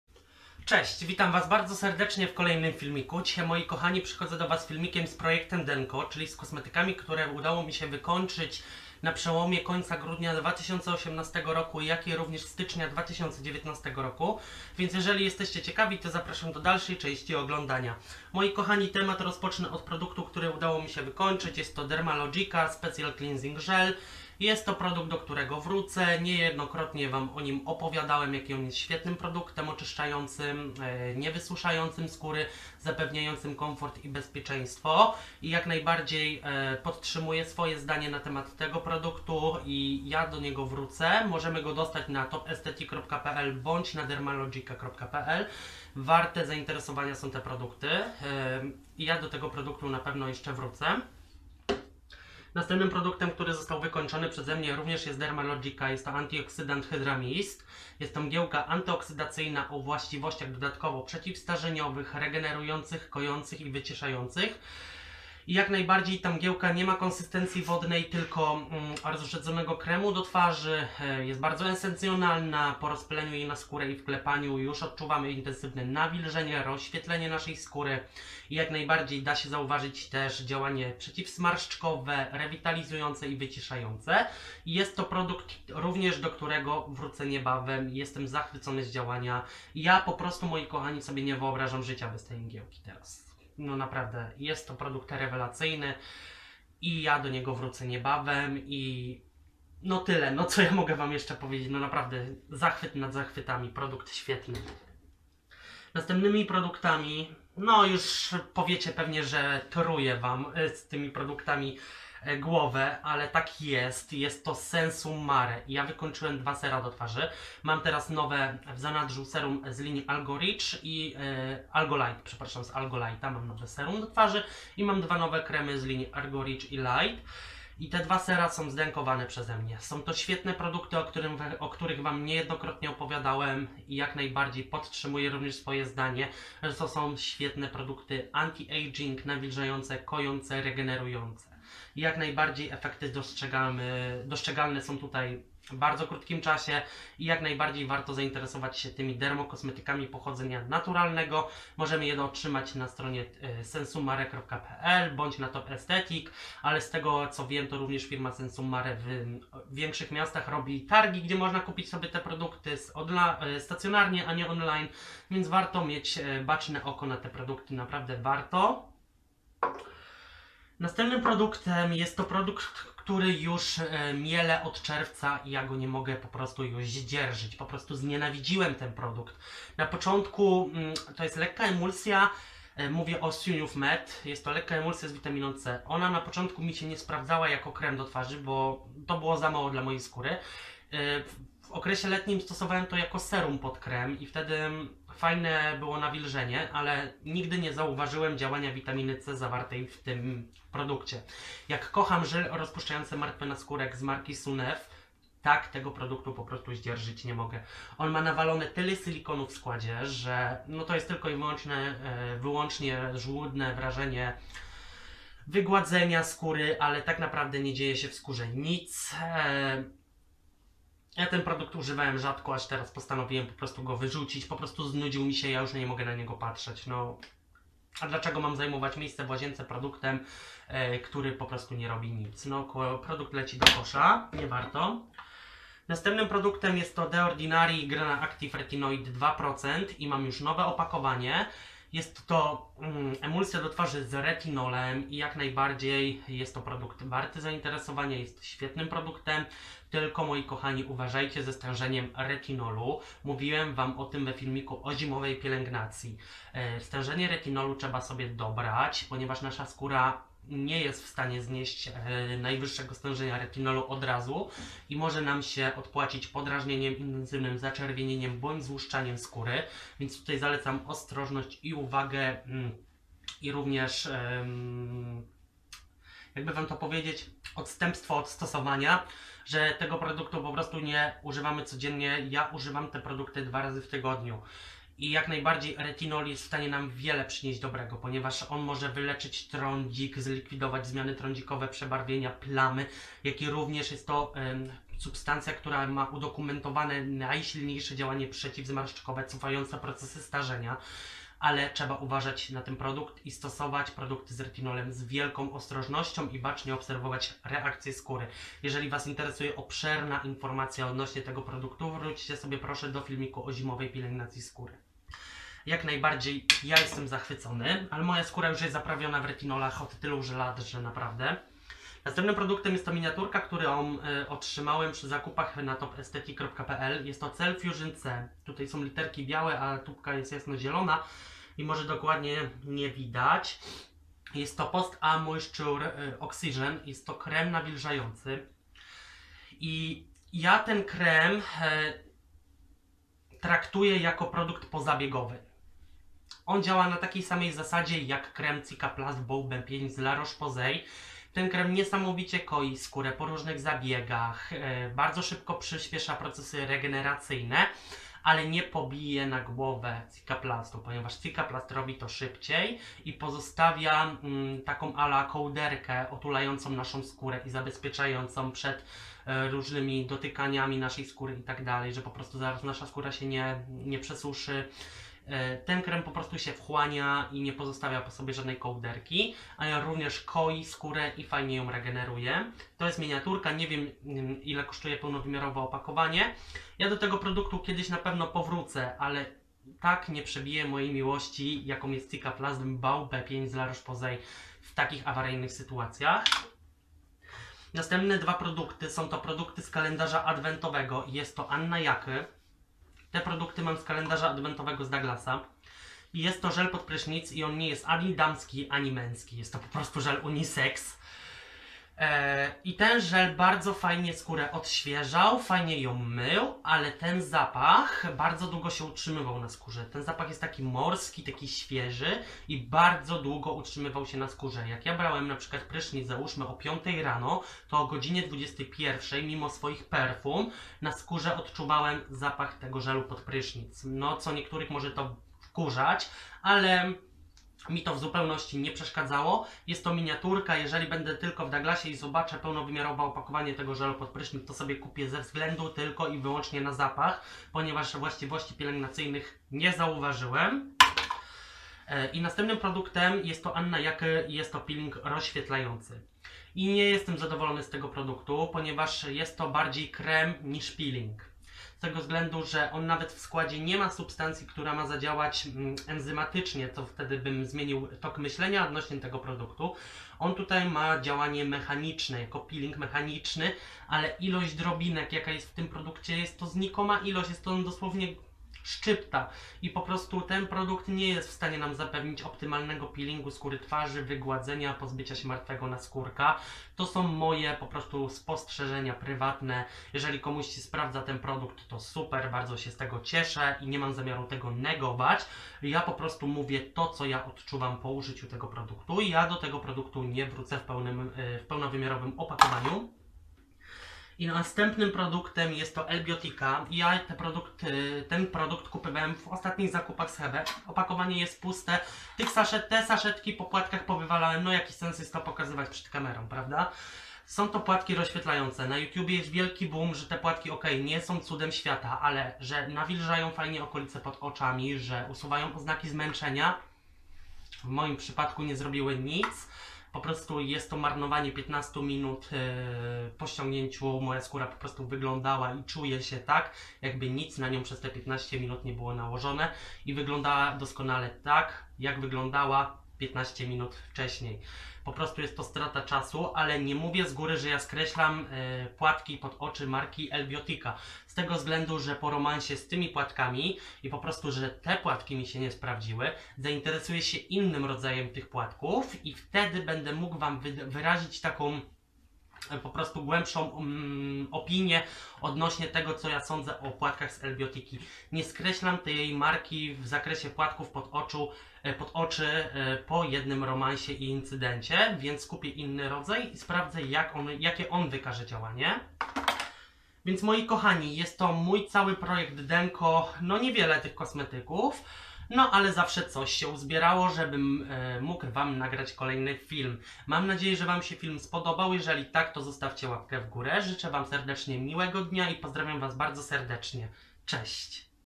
PROJEKT STYCZEŃ - WYKOŃCZONY. :)
Hej, witam was bardzo serdecznie w kolejnym filmie.

Produkty:

Dermalogica special cleansing gel - żel oczyszczający.
Dermalogica antioxidant hydramist - mgiełka antyoksydacyjna.
Sunew med - lekka emulsja z witaminą C.
Sensum mare algorich i algolight - sera do twarzy.
Cell fussion C - moistrue oxygen - krem nawilżający.
Annayake : żel pod prysznic.
Annayake - rozświetlajacy peeling do twarzy.
Lbiotica płatki pod oczy z drobinkami złota.